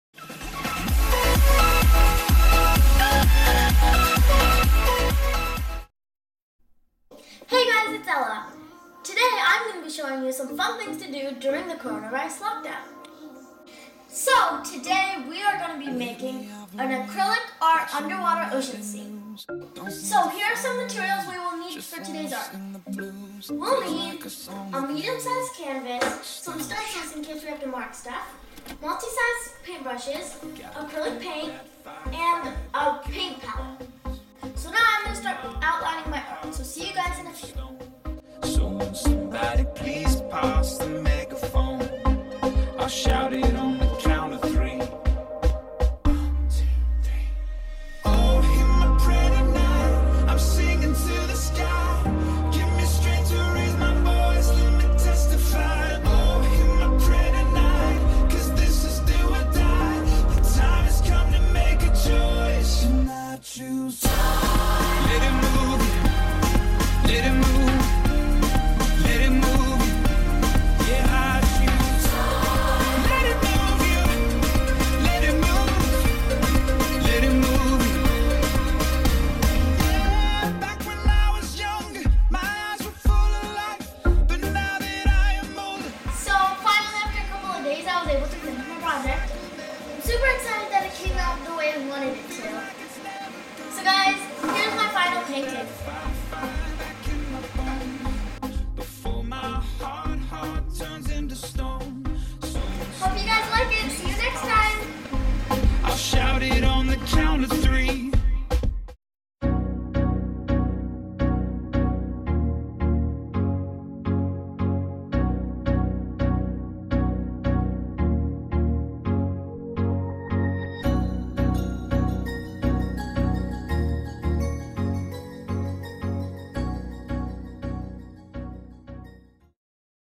World of Art - Lock Down Time Pass
Ella's Canvas painting during the Corona lock down in May 2020.